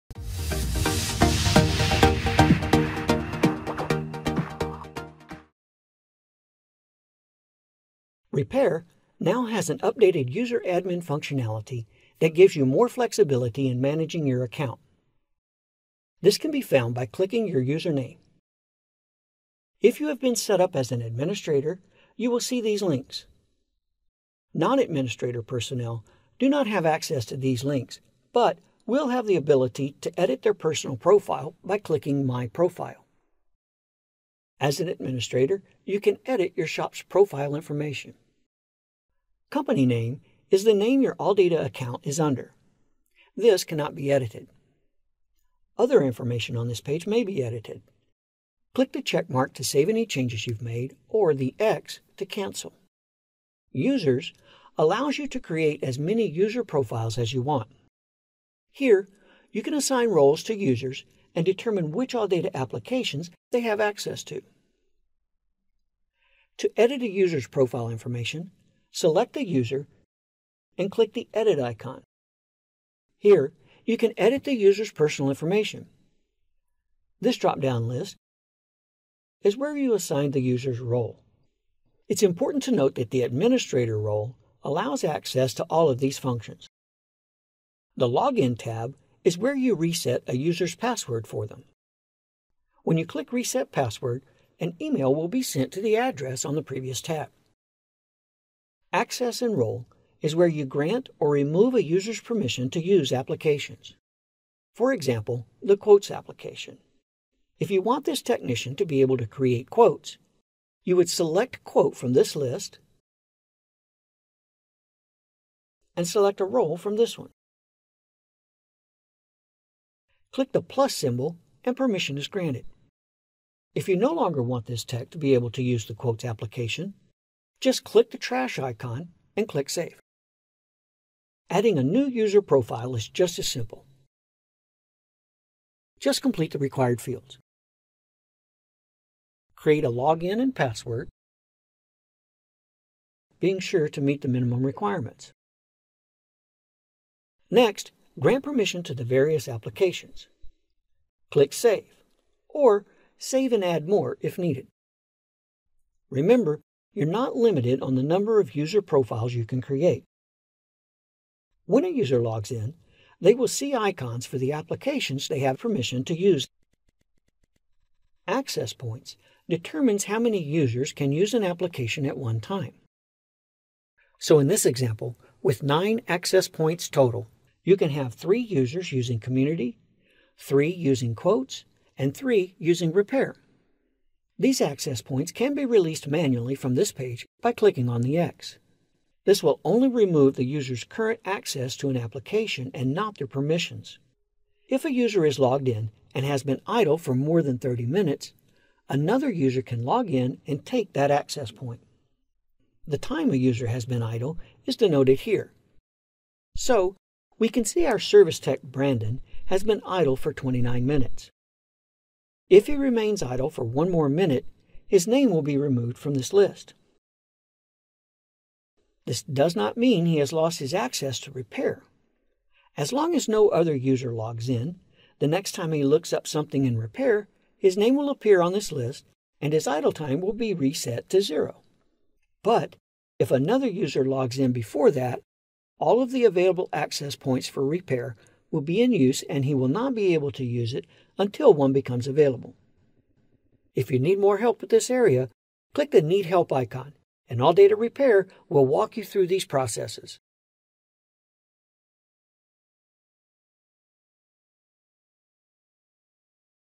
ALLDATA Repair & Collision: User Administration
In this quick tutorial we demonstration how to add users, access points and customize profiles in ALLDATA Repair